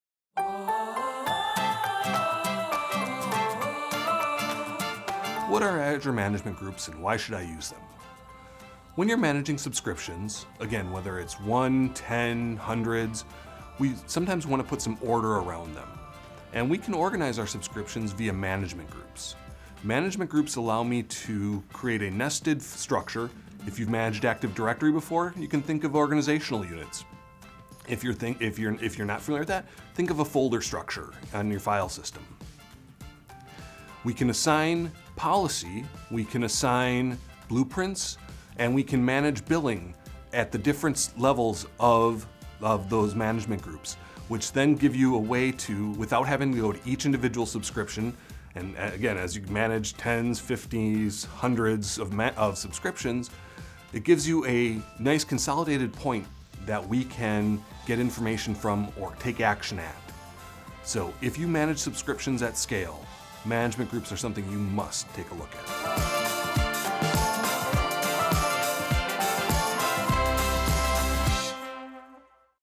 What are Management Groups and why would I use them? | One Ops Question
Tune in to hear Steven Murawski explain what Management Groups are within Azure and why you can use them to make management of your Azure subscriptions better.